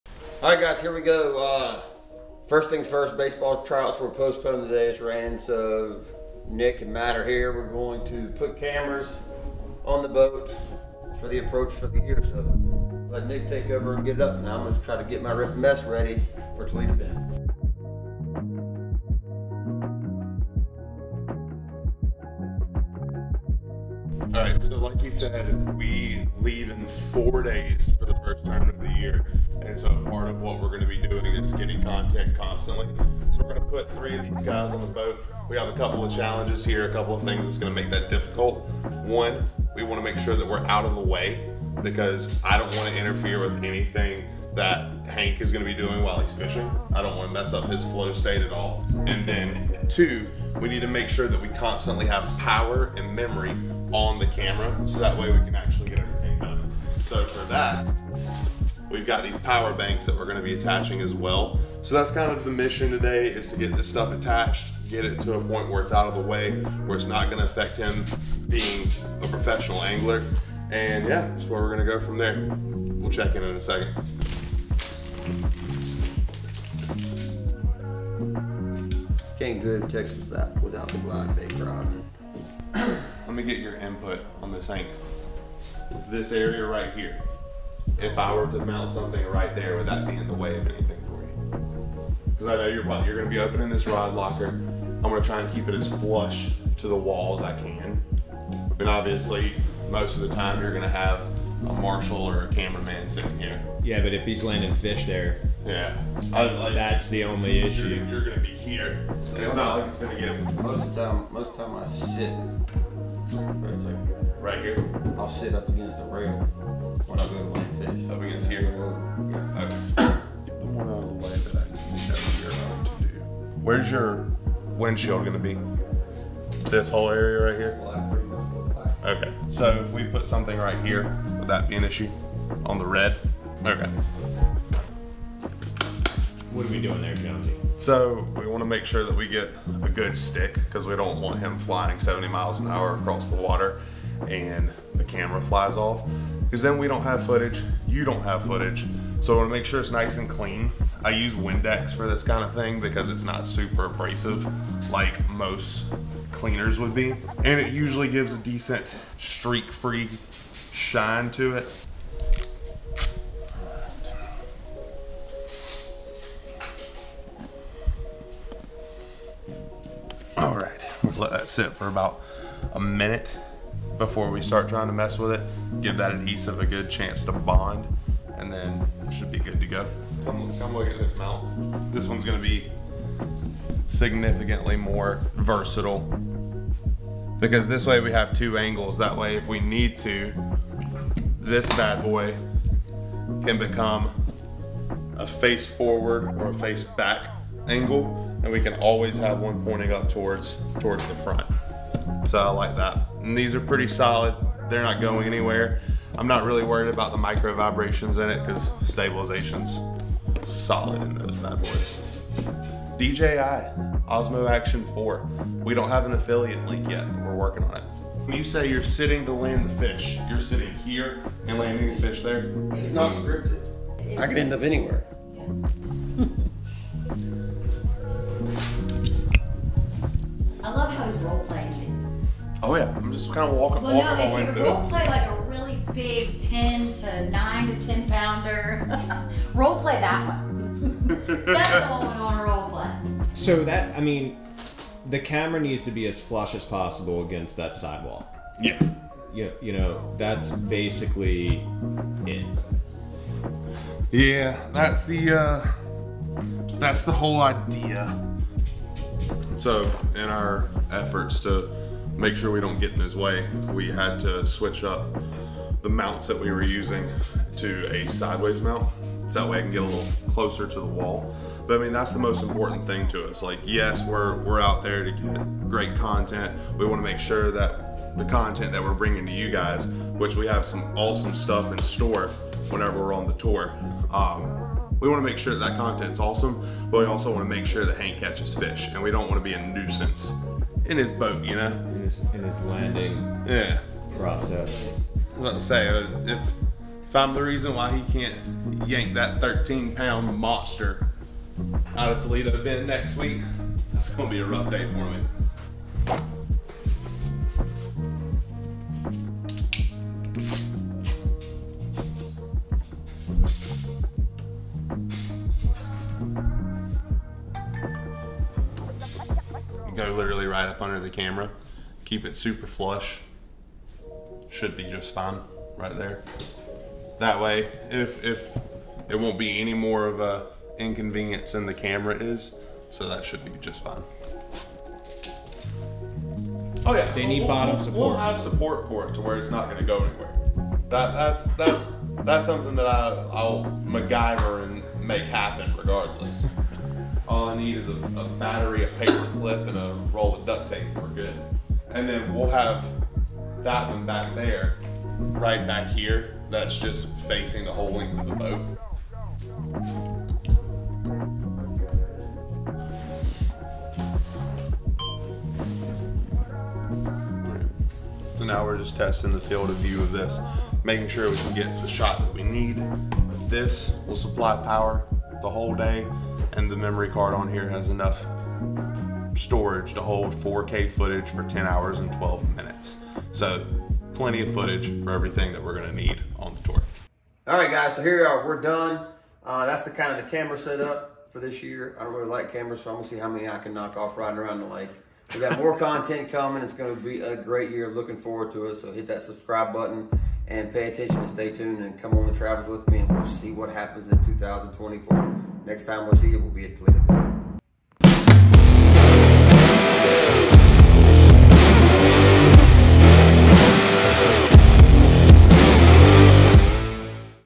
Hank Cherry's Camera Set Up for the BassMaster Elite Series!!
This time we are setting up cameras on the boat! Making sure we can get the best content on the tour!